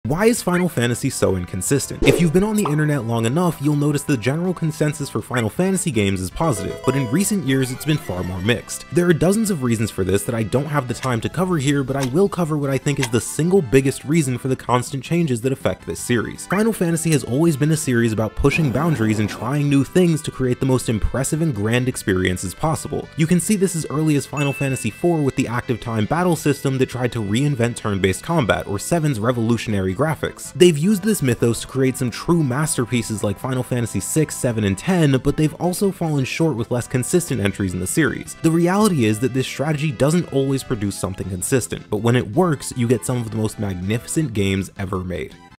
Final Fantasy Is Changing
Final Fantasy is a legendary series home to some of the best games every made, but recently, even with the release of Final Fantasy 16, we've been seeing a more mixed reaction to the series. This short explains why that might not be such a bad thing.